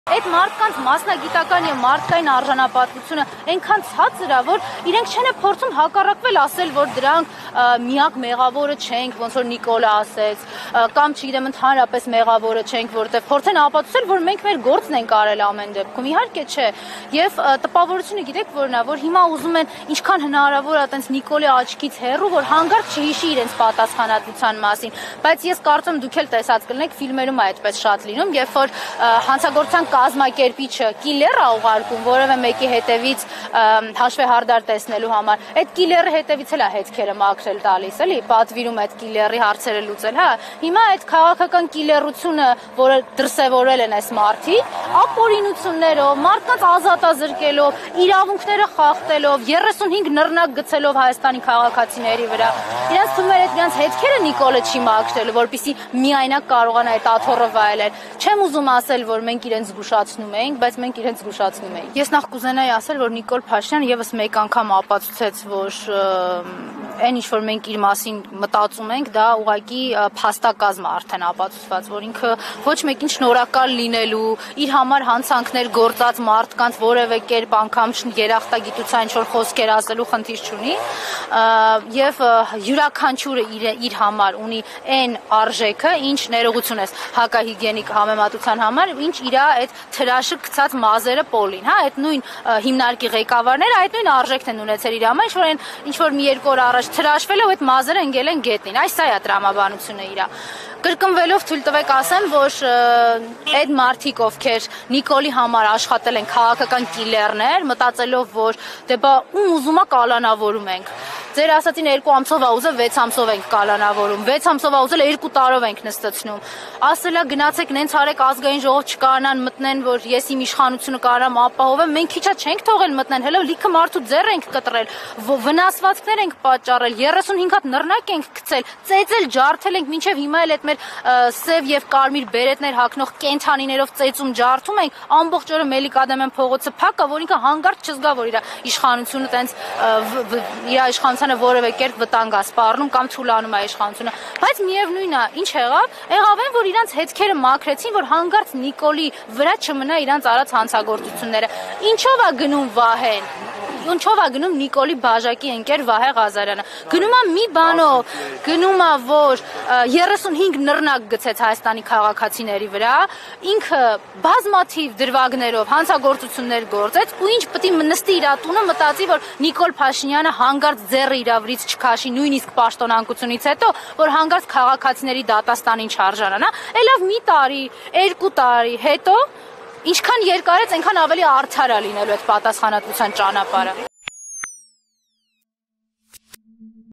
Չեմ ուզում ասել, որ մենք իրենց զգուշացնում էինք, բայց մենք իրենց զբուշացնում էինք. Աննա Մկրտչյան
Բաժանորդագրվեք մեր մյուս YouTube ալիքին՝

Այդ մարդկանց մասնագիտական և մարդկային արժանապատվությունը այնքան ցածր է, որ չեն էլ փորձում ասել՝ «մենք միակ մեղավորը չենք». Պատգամավորը՝ պաշտոնանկություների մասին